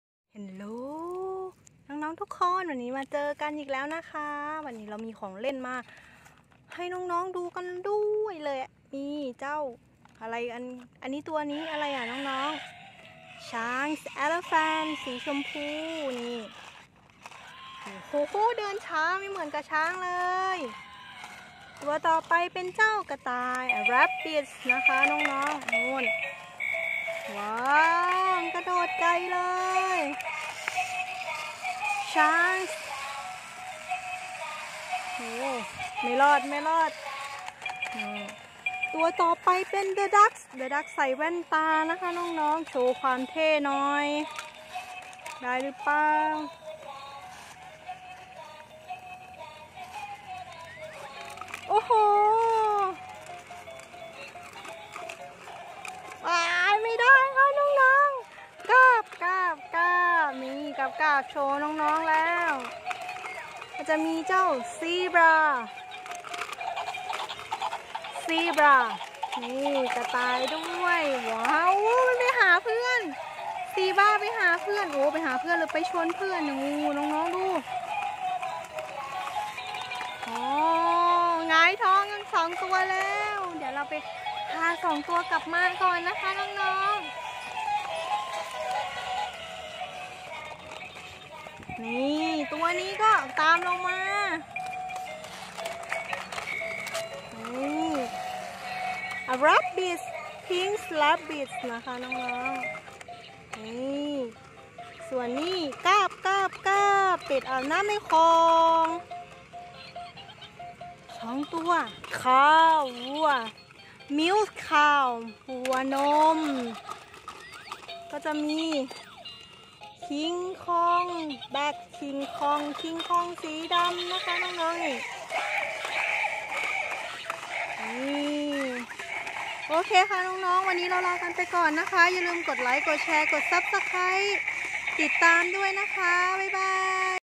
เป็ดใส่แว่นเพื่อนเหล่าสัตว์น้อยใหญ่มาเดินเล่นคิงคองม้าลายกระต่ายช้าง Ducks wearing glasses
เป็ดใส่แว่นเพื่อนเหล่าสัตว์น้อยใหญ่มาเดินเล่นคิงคองม้าลายกระต่ายช้าง Ducks wearing glasses, friends of animals, small and large, come for a walk, King Kong, zebras, rabbits, elephants

ขอบคุณที่เข้ามารับชมคลิปของเรานะครับ